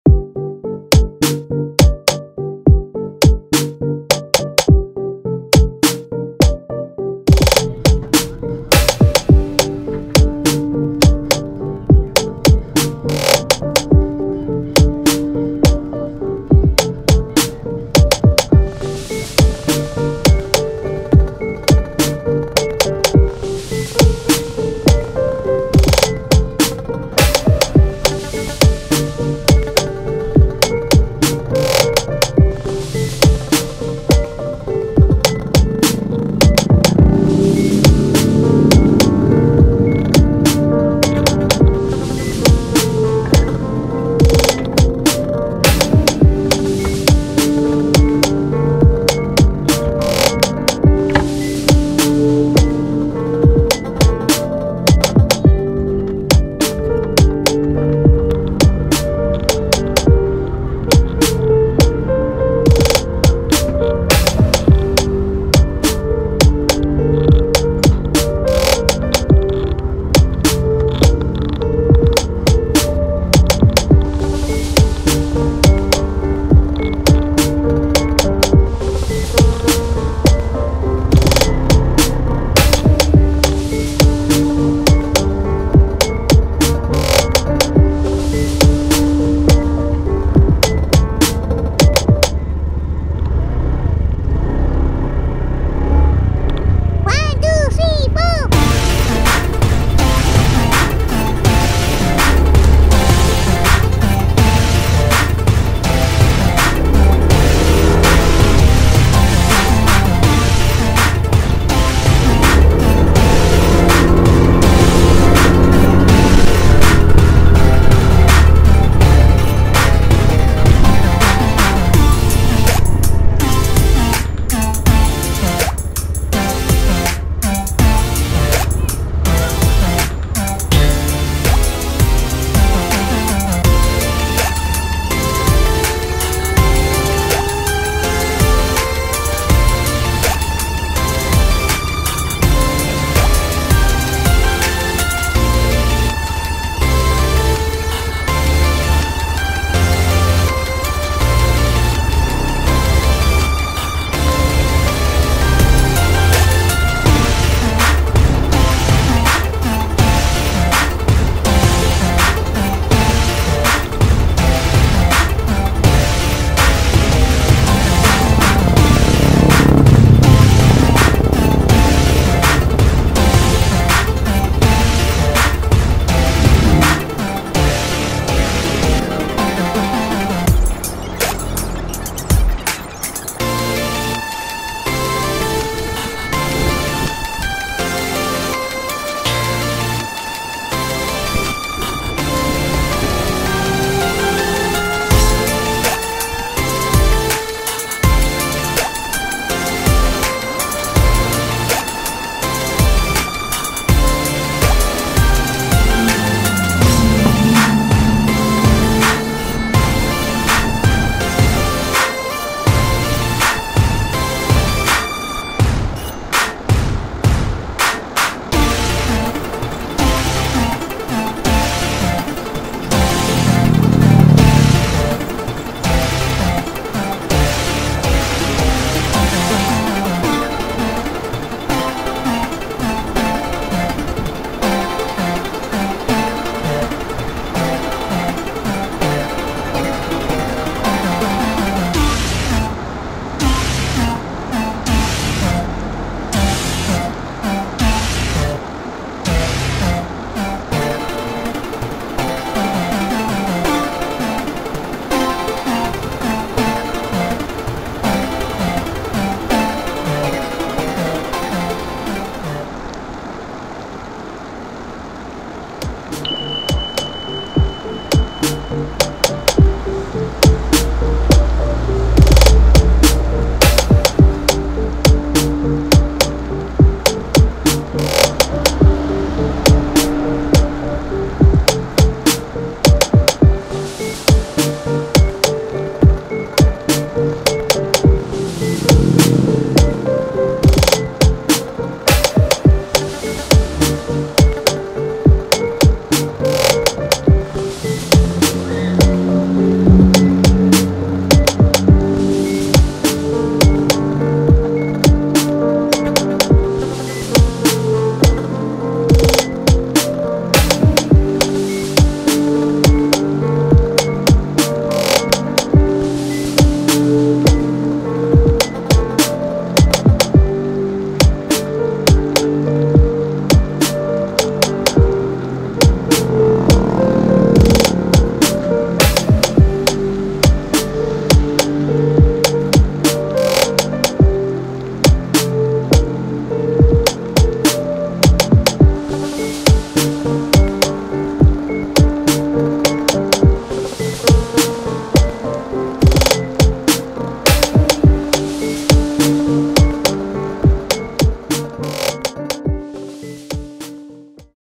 DETIK DETIK JALAN RAYA DI GUYUR HUJAN DERAS
perjalanan terhenti karena turunya hujan deras